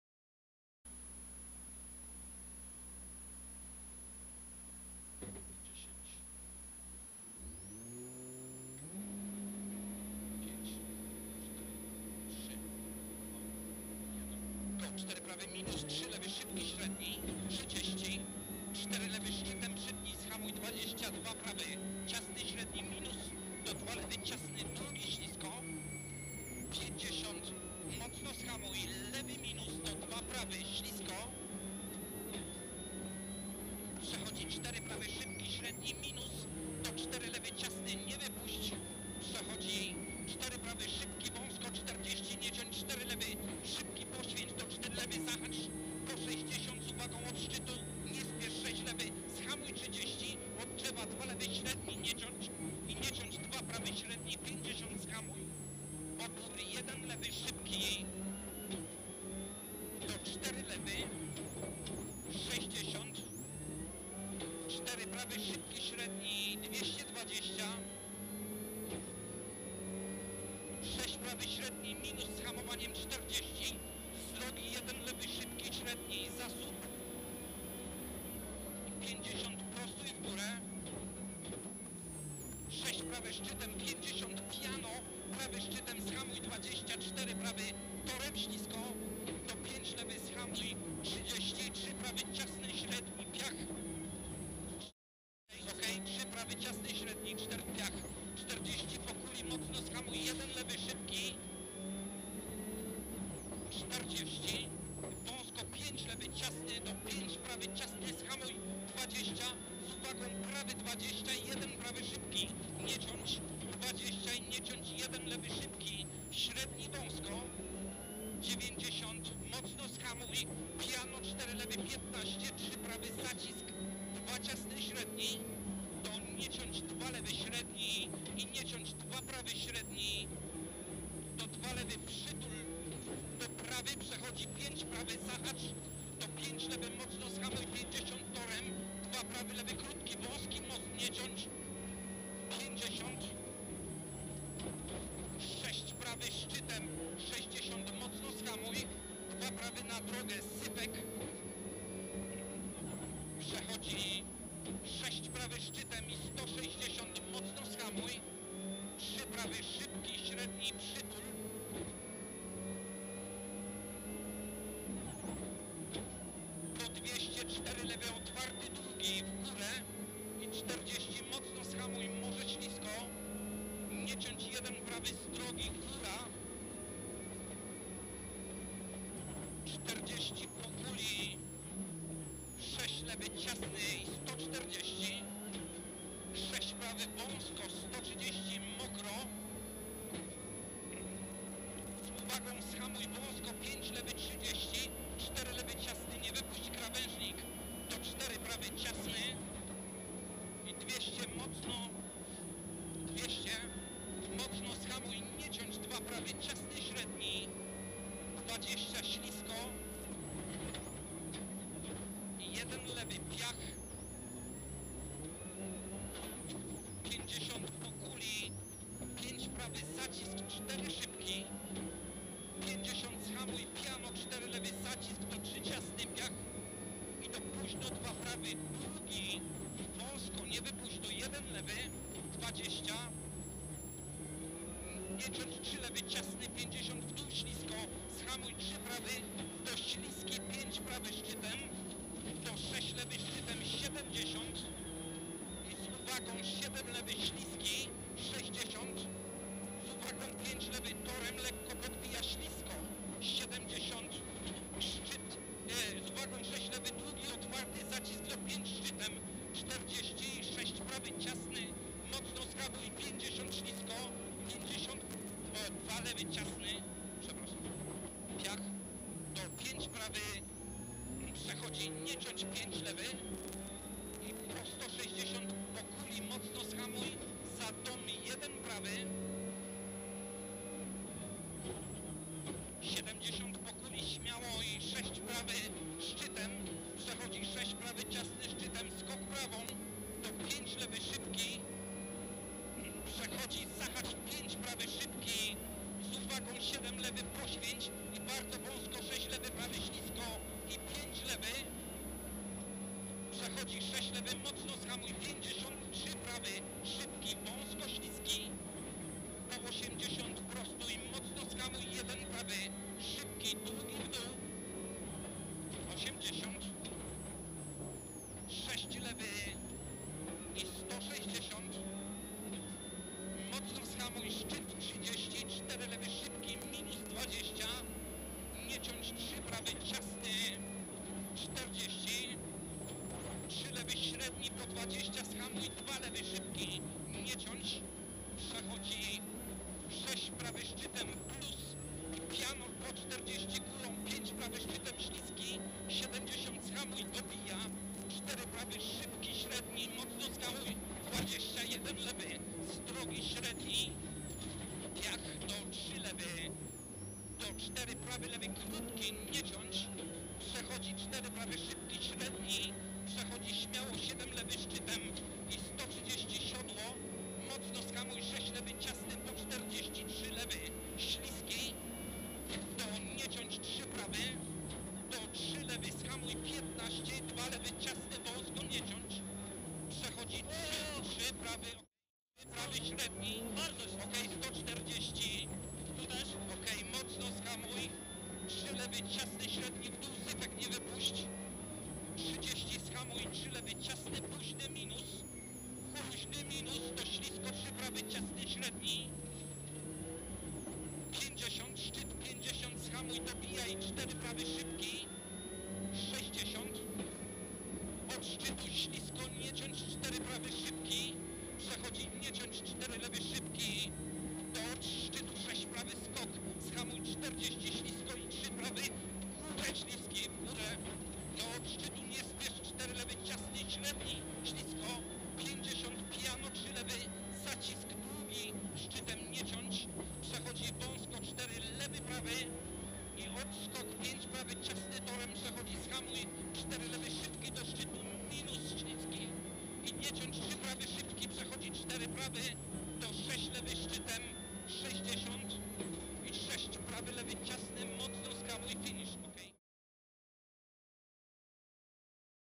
Onboard: Circuit of Ireland 2016 SS9 - Bulls Brook - Kajetanowicz/Baran
This is onboard footage from Circuit of Ireland special stage number 9 - Bulls Brook (12,19 km) - one of the three stages in this event won by LOTOS Rally Team crew.

Onboard z oesu Bulls Brook (12,19 km) - dziewiątej próby Rajdu Irlandii 2016 i jednego z trzech odcinków tego rajdu, na których załoga LOTOS Rally Team była najszybsza.